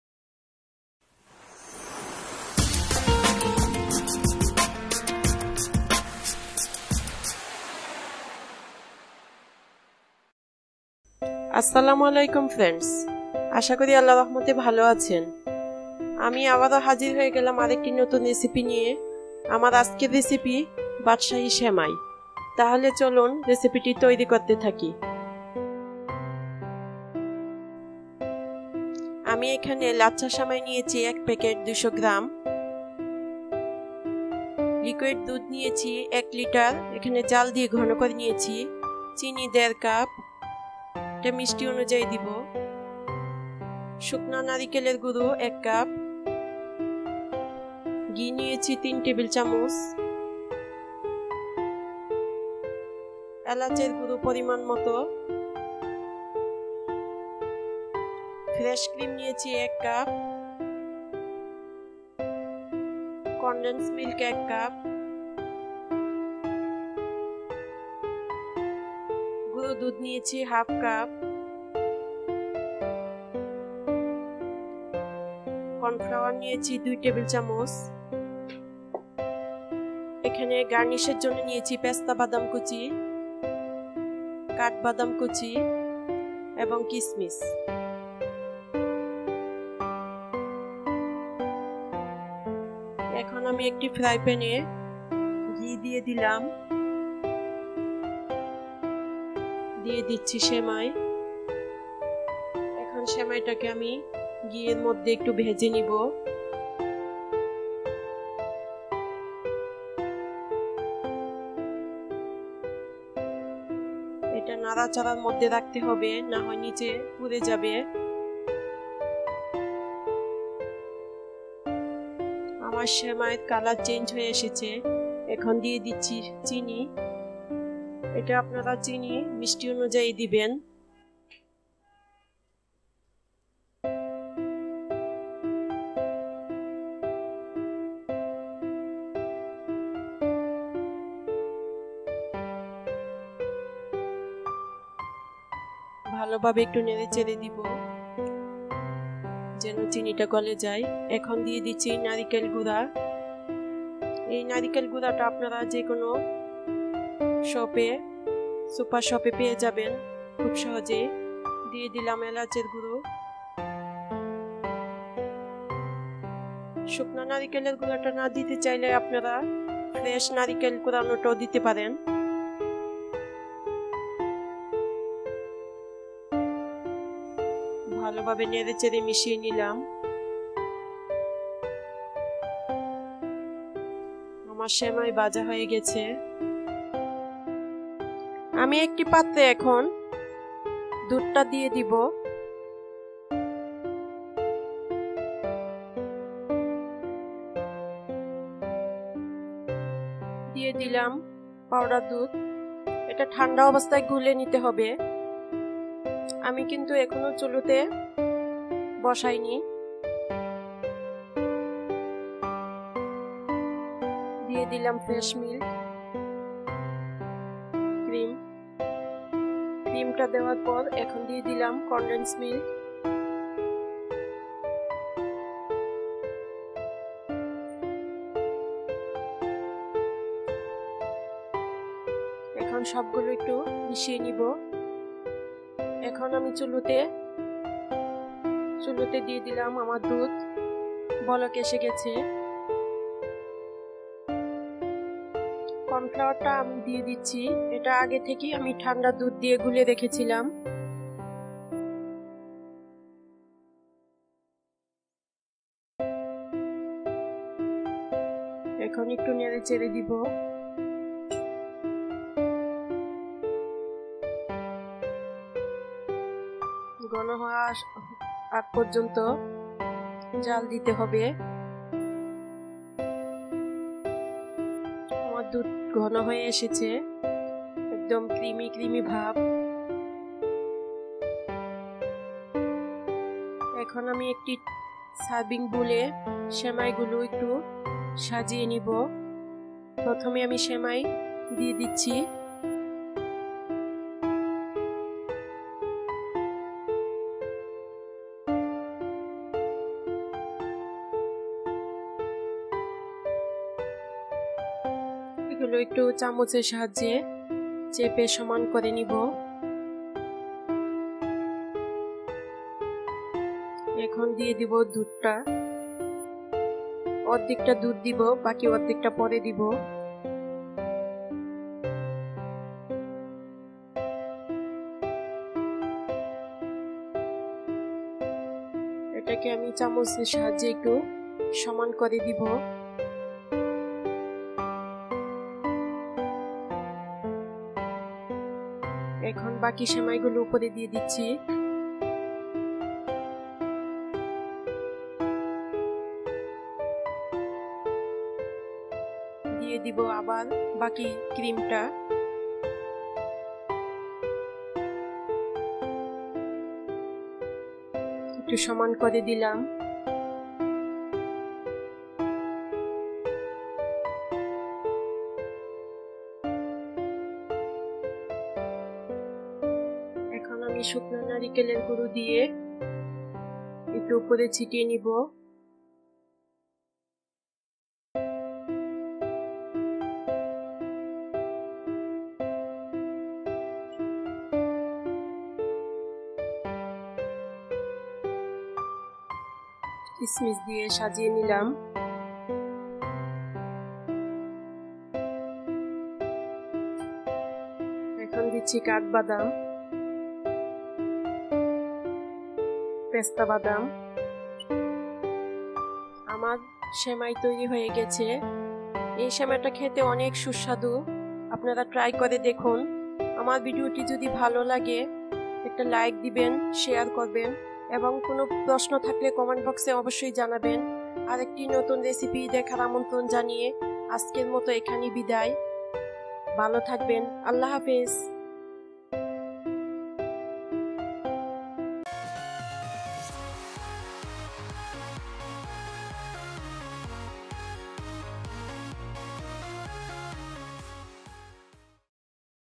বাদশাহী সেমাই রেসিপি // Badshahi Milky Cream semai recipe ..
উপকরন

*১ পেকেট সেমাই (২০০ গ্রাম )
*১ ১/২ কাপ চিনি
*১ কাপ নারকেল গুড়া
*১ লিটার দুধ
*১ কাপ ফ্রেস ক্রিম
*১ কাপ কনডেন্স মিল্ক
*১/২ কাপ গুড়া দুধ
*২ টেবিল চামচ কনফ্লাওয়ার
*১/২ চা চামচ এলাচ গুড়া
*৩ টেবিল চামচ ঘি
*কাঠ বাদাম,পেস্তা বাদাম,কিসমিস সাজানোর জন্য

আমার এই  ভিডিওটি  ভালো লাগলে অবশ্যই লাইক | কমেন্ট এবং শেয়ার করবেন ।